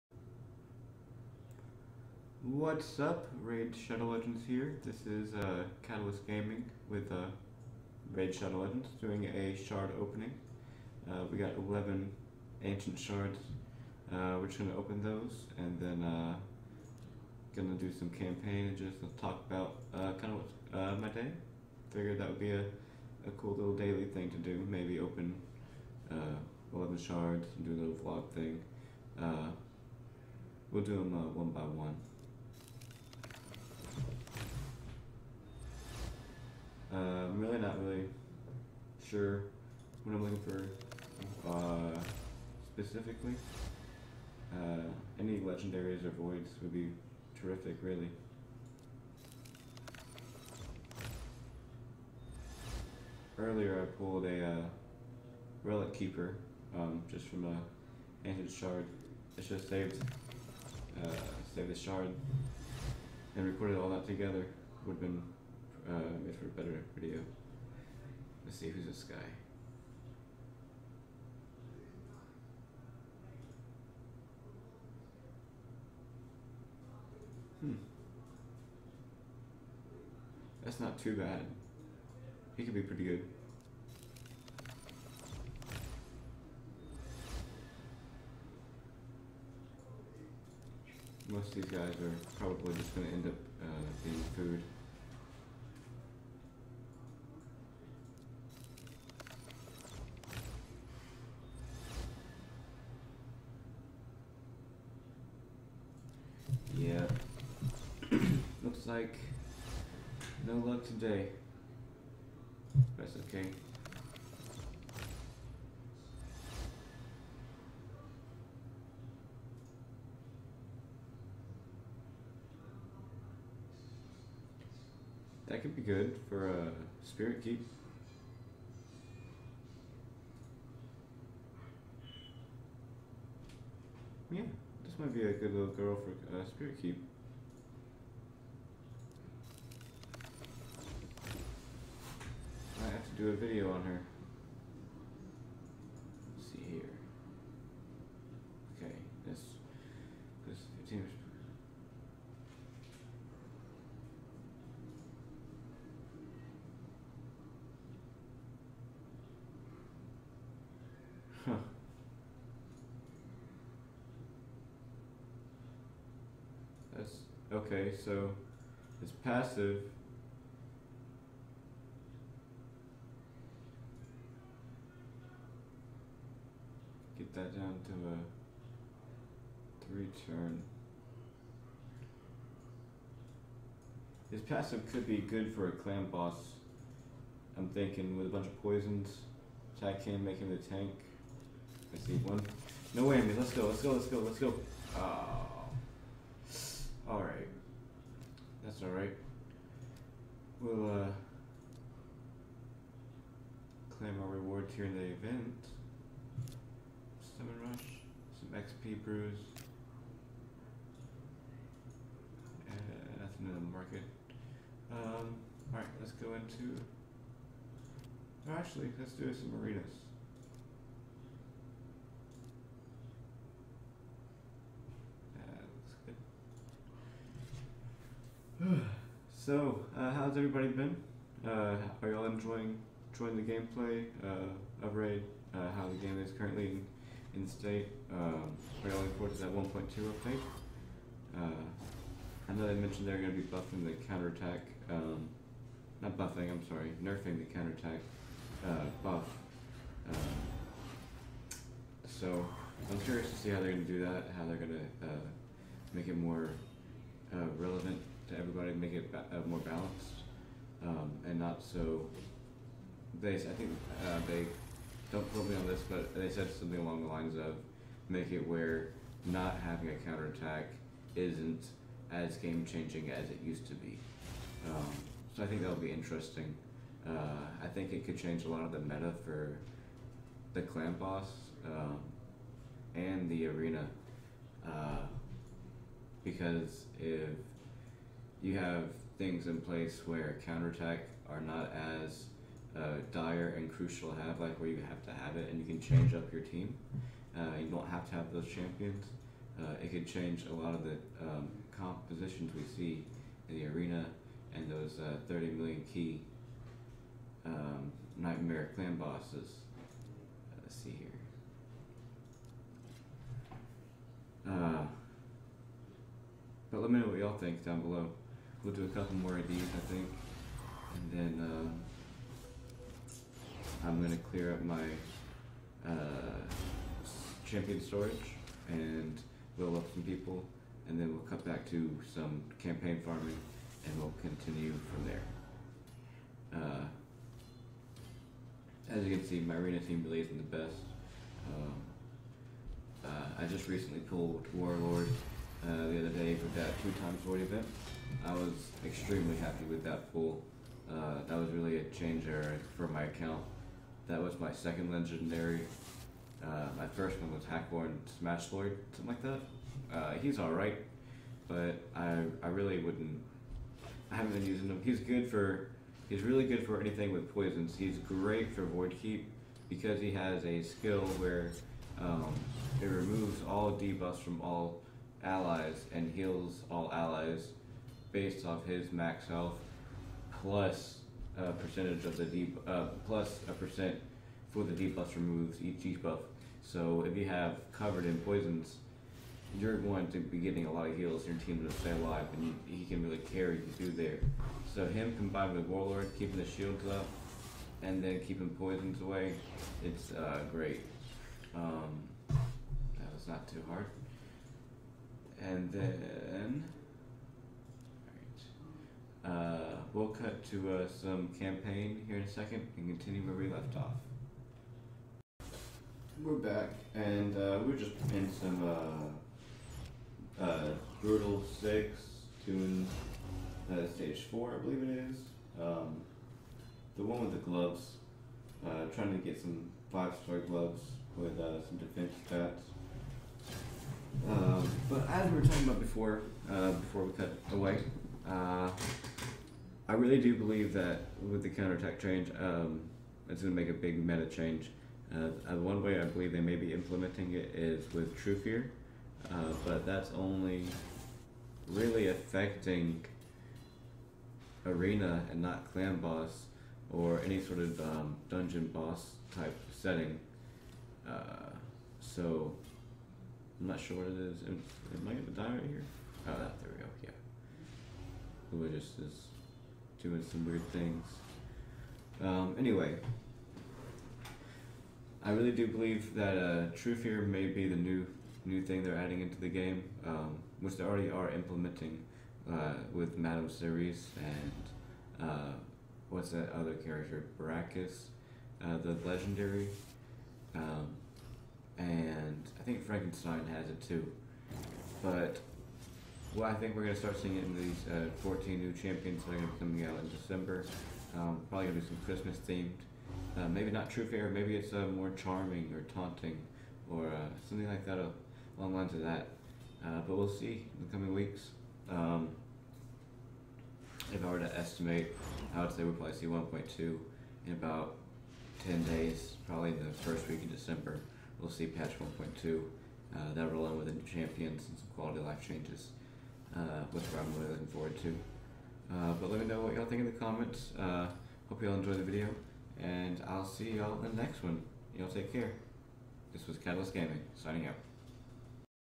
Ancient Shard Opening! |Talking about the state of the CA meta
Good morning everyone! Today we are opening 11 ancient shards and then we are talking about the current state of the games meta with counterattack.